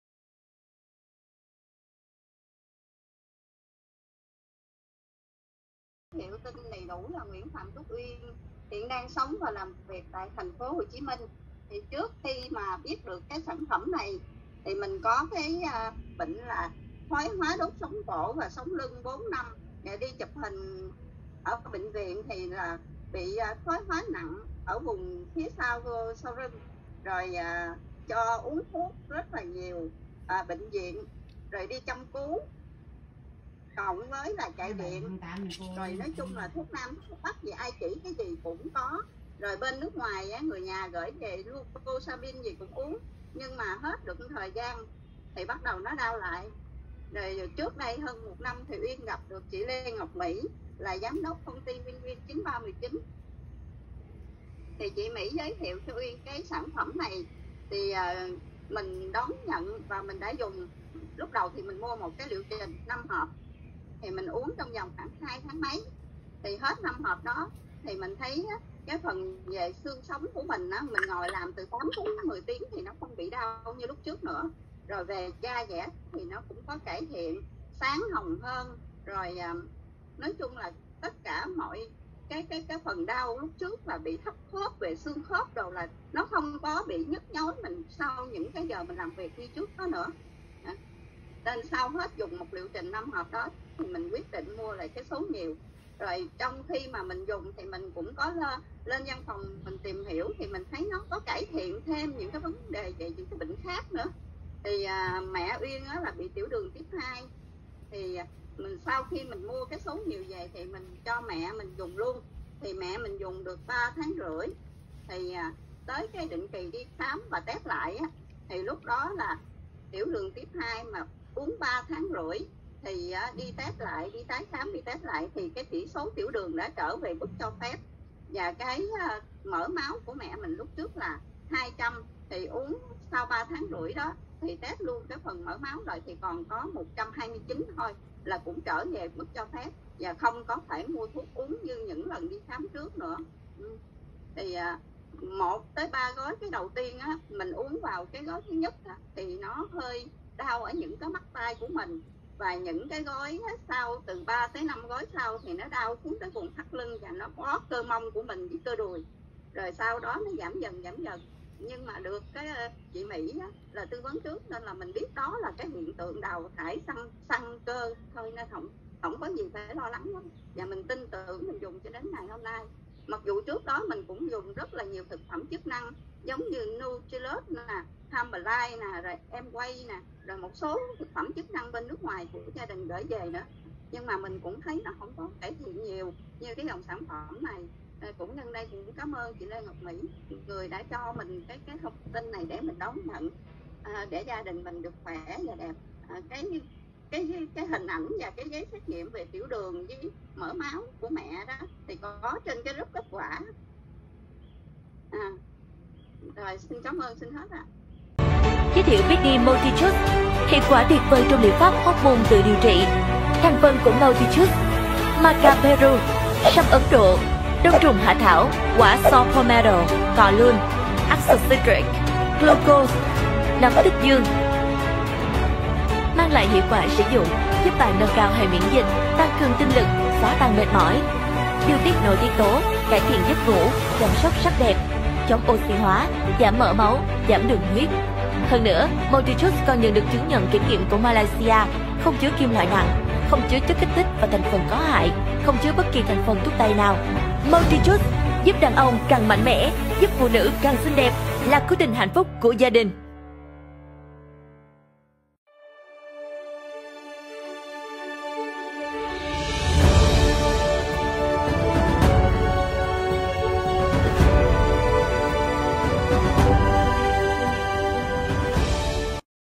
Nguyên nhân gây bệnh & cách hoá giải siêu thị bệnh tật sau khi dùng Tế Bào Gốc MULTI JUICE.
Nguyên nhân gây bệnh & cách hoá giải sieu thi bệnh tật sau khi dùng Tế Bào Gốc MULTI JUICE.